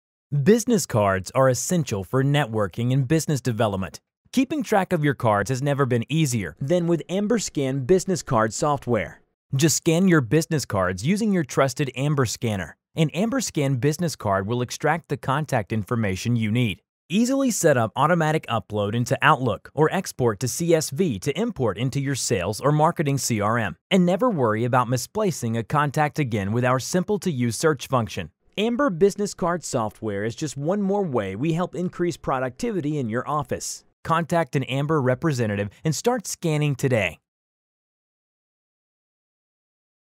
Increase Productivity with AmbirScan Business Card Software
Business development is hard, but staying organized doesn’t have to be. Keeping track of your business cards has never been easier with AmbirScan Business Card Software. Paired with your trusted Ambir scanner, this software digitizes your card collection and automatically imports contacts to Outlook or exports to .CSV for easy upload into your sales or marketing CRM.